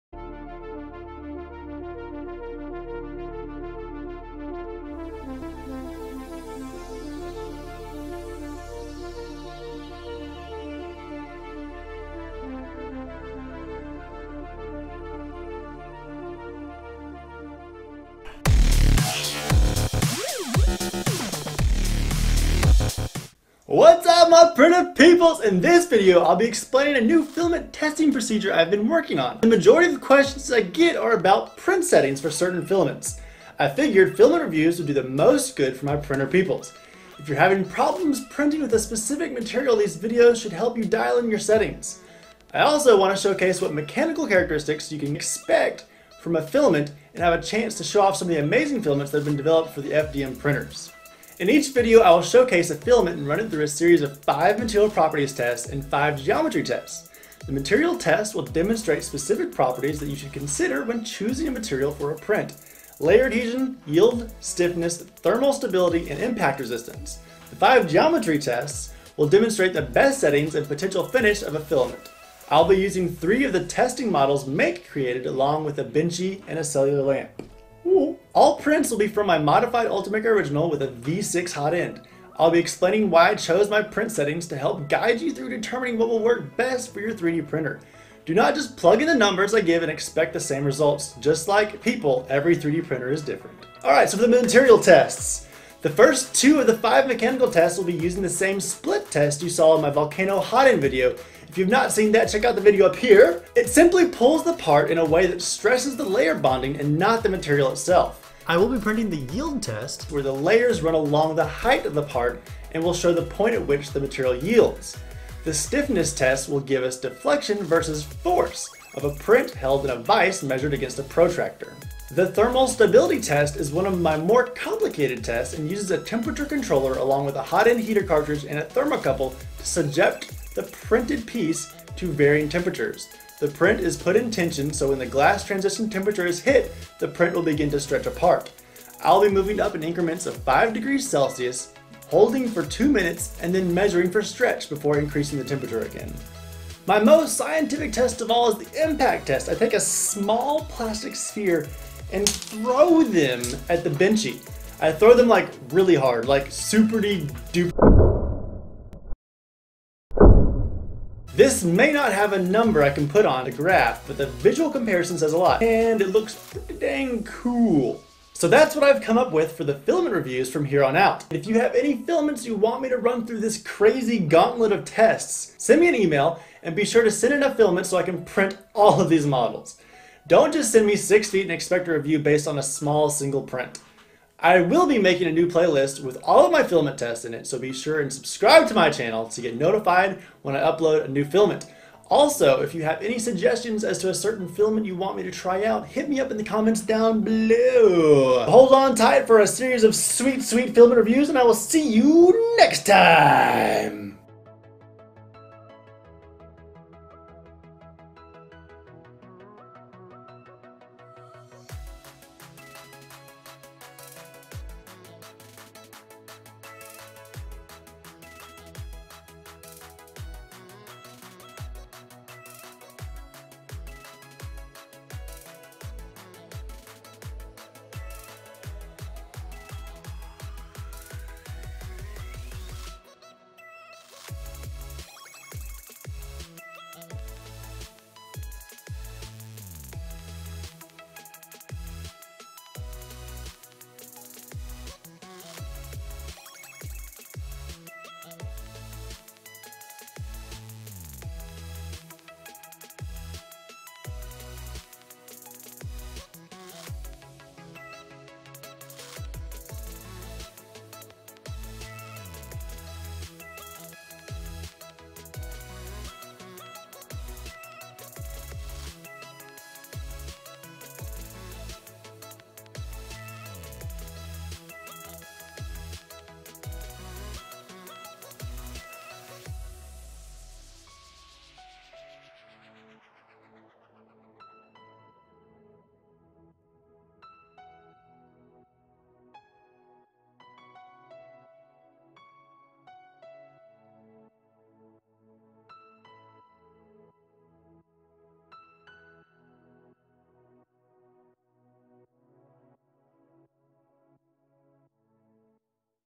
Filament Testing Methods
I want to start sharing more information about the filaments I use and what makes them special. To accomplish this I have come up with 5 material properties tests and 5 geometric tests that help fine-tune print settings and showcase what material properties each filament offers. Using these tests I will not only be able to give you the best print settings but I will also be able to show you what the material properties are like.

All prints are printed with 2 perimeters and 25% infill
*Except the stiffness model. It is printed 2 perimeters 80% infill*

MATERIAL PROPERTIES TESTS:
Layer Adhesion - Split model printed oriented standing up (T on buildplate). Force measured while pulling until failure.

Yield Strength - Split model printed oriented laying on side (Yield on buildplate). Force measured while pulling until failure.

Stiffness - Stiffness model held with vice and force vs angle displacement measured.

Thermal Stability - Thermal model loaded in tension with heater cartridge in middle. Temperature is held for 2 minutes then measured for stretch before increasing temperature additional 5 degrees.

Impact resistance - Benchy model being hit with .25 gram airsoft pellets at 400 fps

GEOMETRY TESTS:
Bridging - Make's bridge model is a great test for seeing a material's ability to stretch and to ensure your bridging settings are working.

Small Feature - Make's Magazine fine positive feature model is a retraction stress test as it tests retraction and temperature settings.

Overhang - Make's overhang model pushes the limits of any 3d printer with varying angles of an unsupported beam. A material that easily warps will not pass this test.

 Benchy - Because Benchy

Cellular Lamp - When scaled down this is a very hard print that demands filament not warp, many  retractions, has lots of small features, and everything is an overhang. The ultimate stress test.

LINKS
Thingiverse models:
My test models